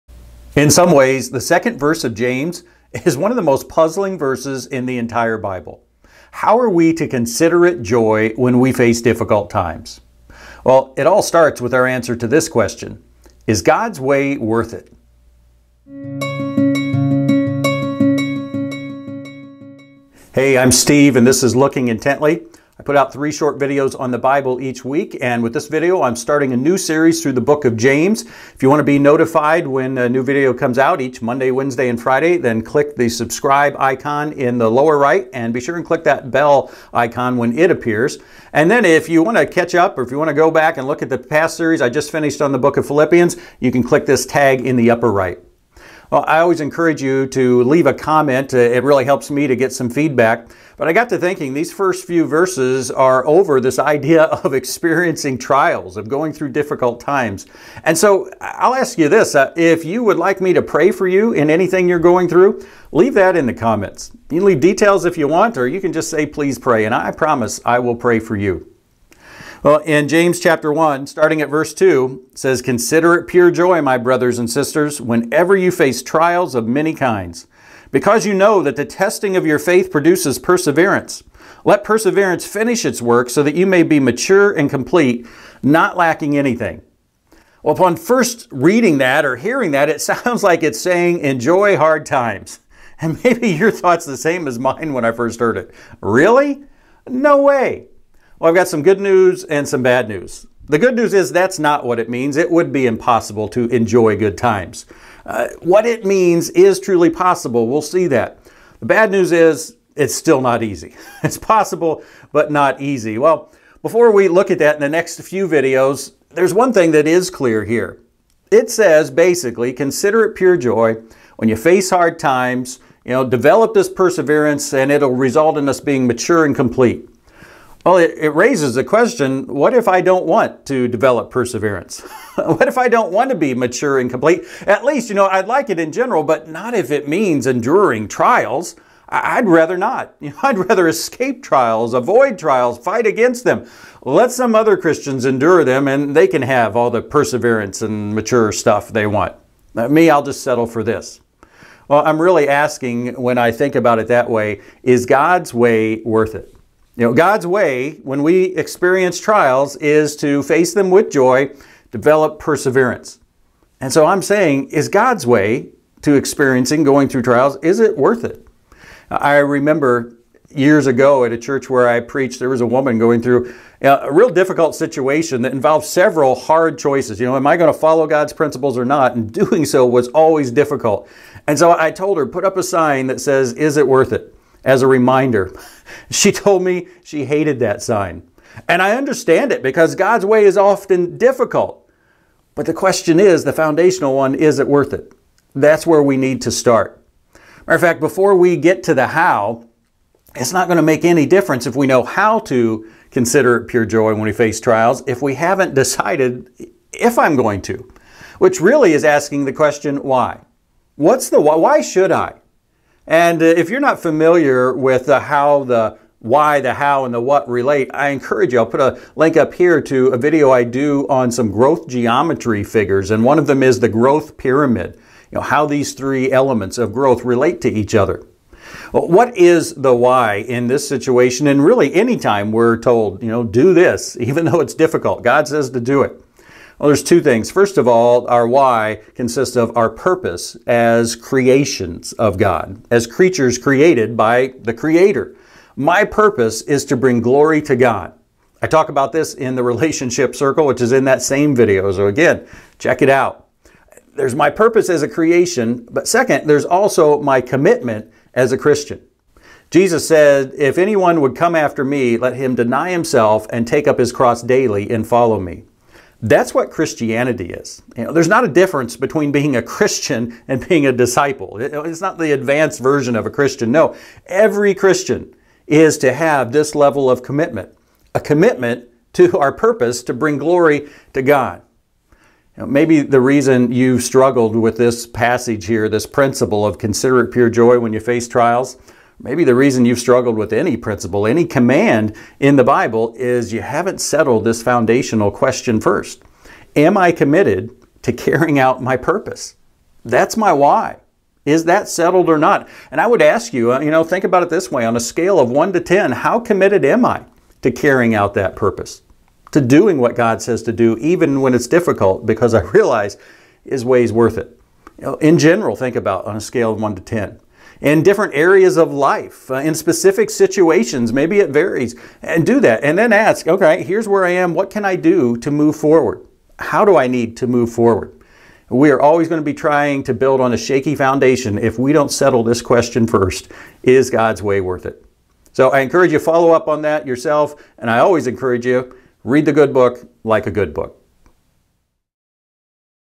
Is God's Way Worth It? (James - Video 1)
James says it is possible to face trials of many kinds with joy. Yes, it is possible to consider it all joy (video) when we face trails of various kinds, BUT we must first ask: Is God’s way worth it?

See lessons from the book of James for living the Christian life and growing spiritually as a follower of Christ.

Part of a series of short video Bible study lessons on the Bible book of James.